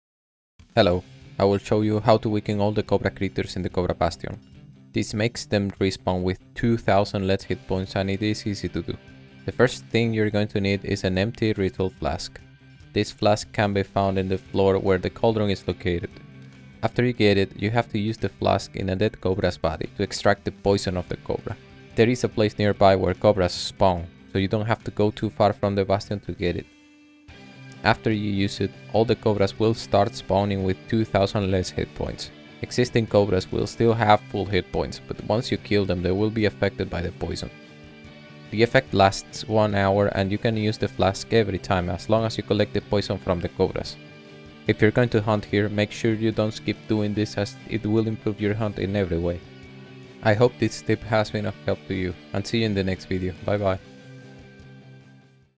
How to spawn COBRAS with 2000 LESS HP
This is a detailed guide on how to make Cobras lose 2000 HP every time they respawn...